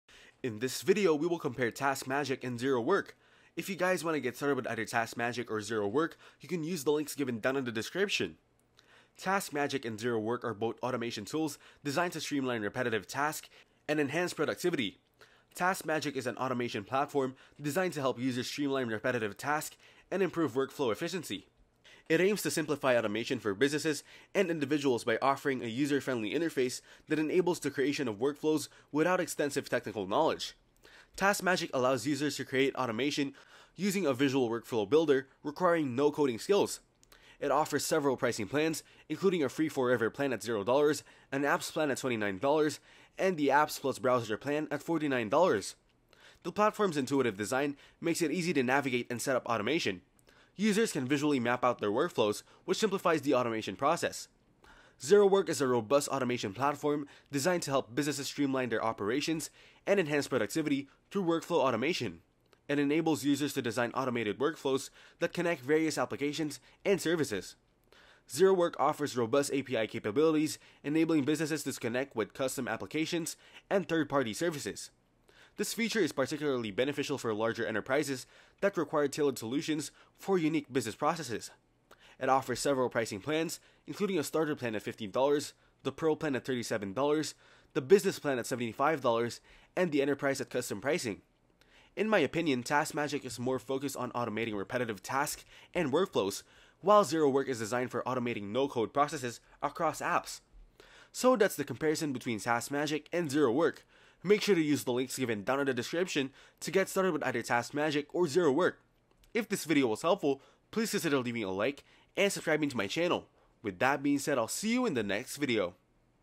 ZeroWork vs TaskMagic (2026) | Which One is Better?
In this video I'll compare ZeroWork vs TaskMagic.